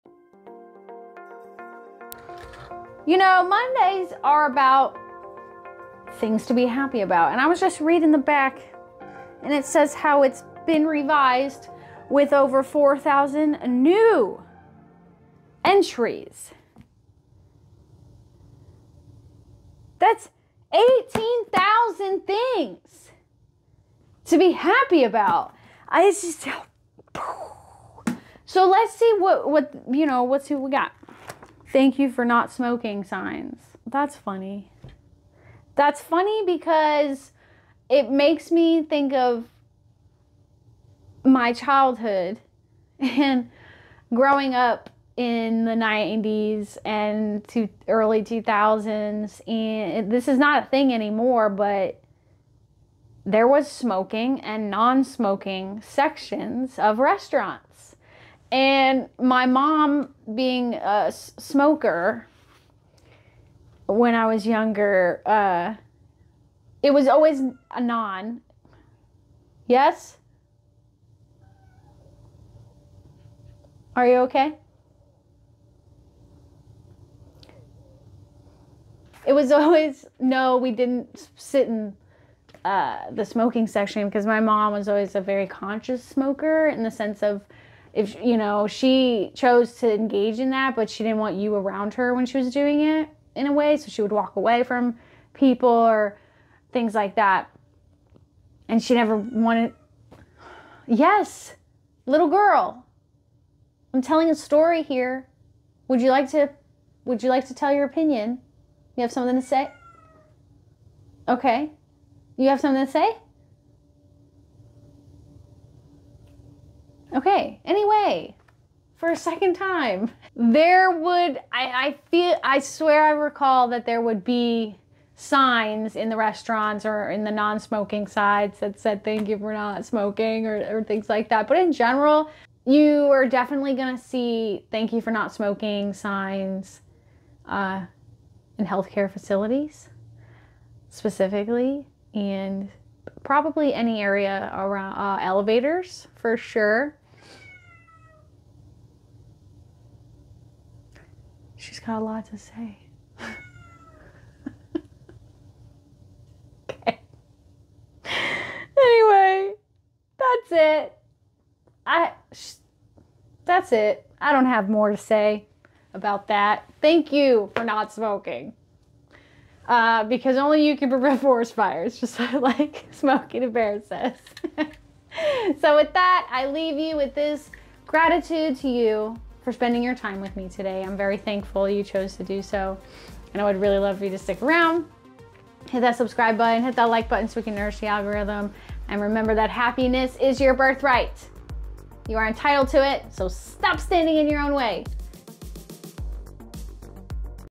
Thank You For Not Smoking | 14,000 THINGS EP. 33
🥀Hey I’m Rose! On this episode of 14,000 Things To Be Happy About it’s a bit of a weird one. Strange, frankly. However, someone out there is genuinely happy and grateful for the signs reminding all to not smoke in areas that you shouldn’t. We all know what happened to Zoolander’s friends as the gas station.